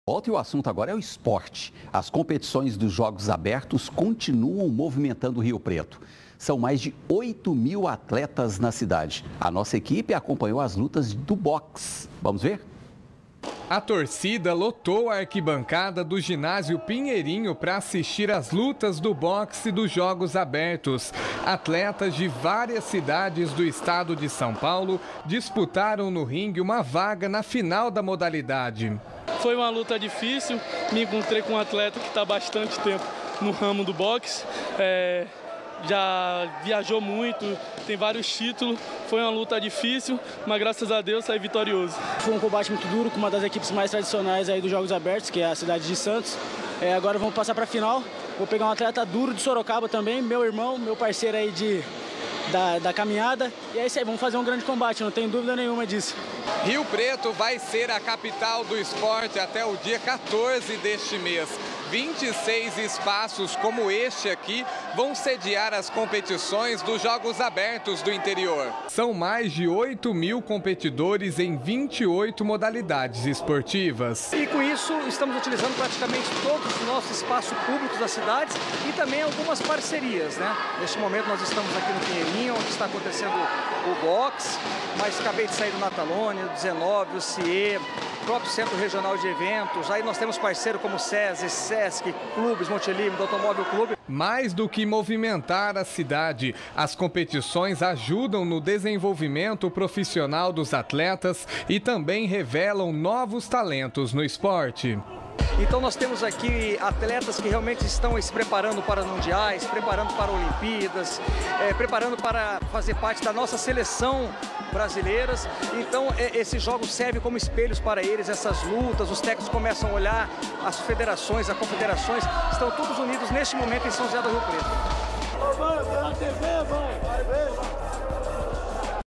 Jogos Abertos: Lutas de box movimentam ginásio de Rio Preto
As competições dos jogos abertos continuam movimentando Rio Preto, são mais de oito mil atletas na cidade.